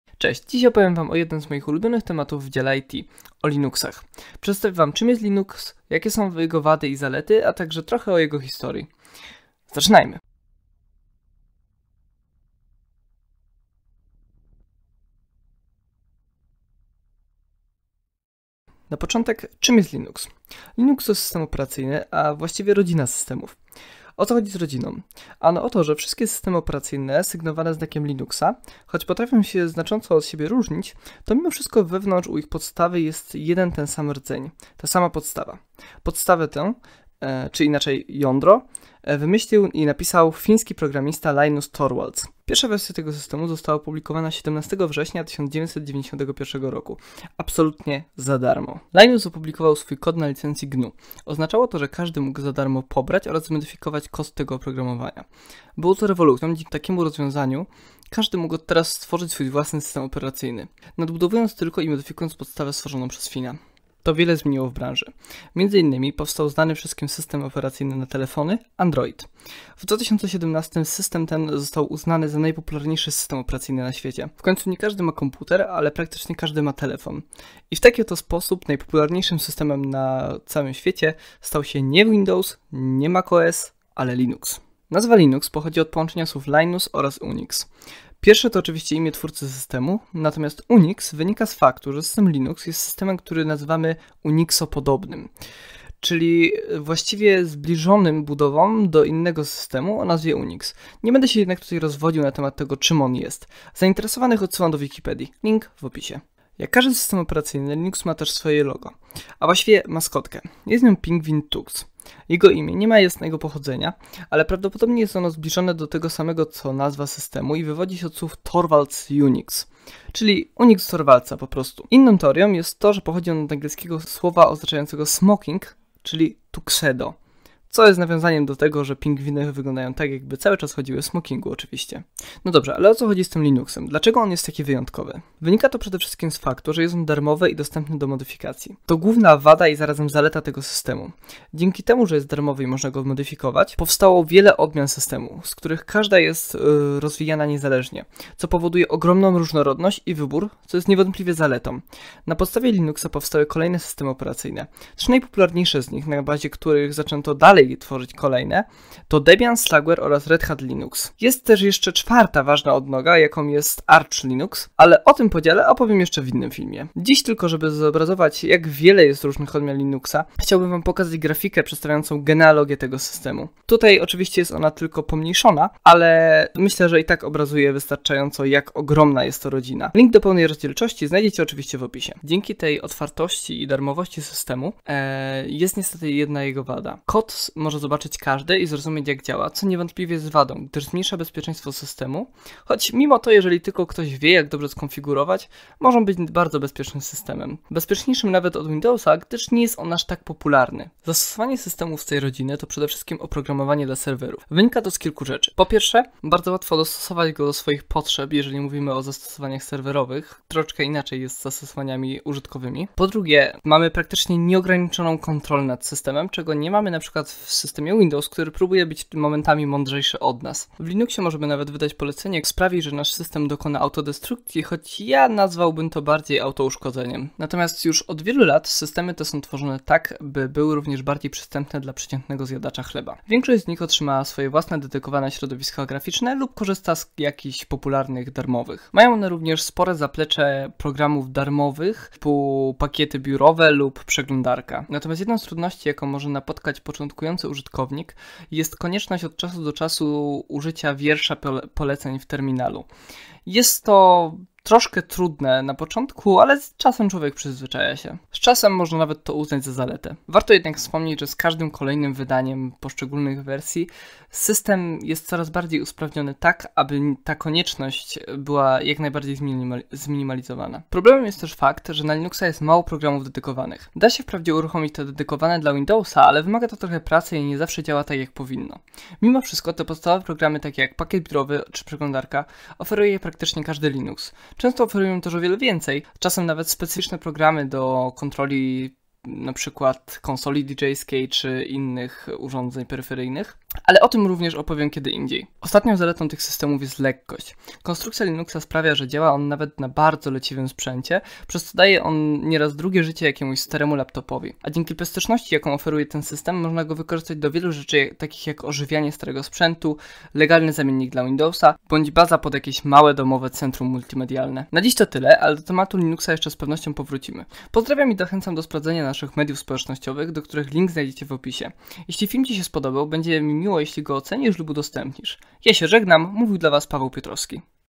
System operacyjny Linux — geneza, historia, możliwości wykorzystania
Na pewno słyszałeś kiedyś o systemie operacyjnym Linux. Jeśli chcesz dowiedzieć się więcej o rodzinie darmowego i otwartego oprogramowania operacyjnego, jego historii, genezie i możliwościach wykorzystania Linuxa, obejrzyj nowy film na kanale YouTube Klubu Kodowania MBP w Gorlicach. Podcast prowadzi jeden z założycieli Klubu — Paweł Piotrowski.

Zapraszamy również na nasze social media :D